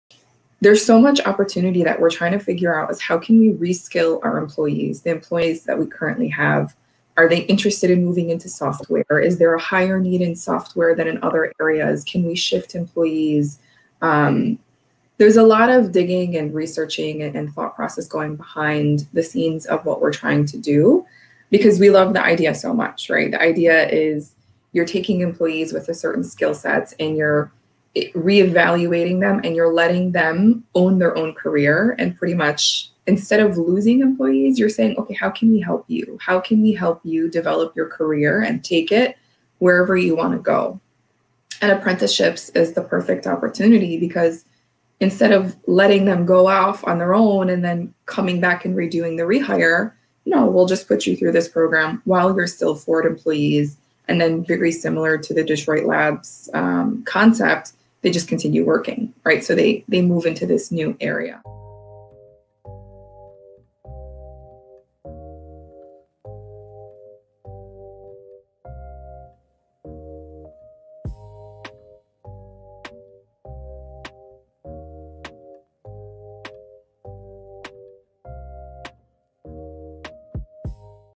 #4: Providing Internal Opportunity Through Software Apprenticeships
Taharah Saad, Employee Development Program Manager at Ford, shares how Ford is leveraging apprenticeships, which includes giving employees autonomy over their development and career growth.

On this episode of Labs Live, Dan and guest host Erika Languirand, Director of People Development at Labs, lead a panel discussion about the value and role of apprenticeships in this modern software industry.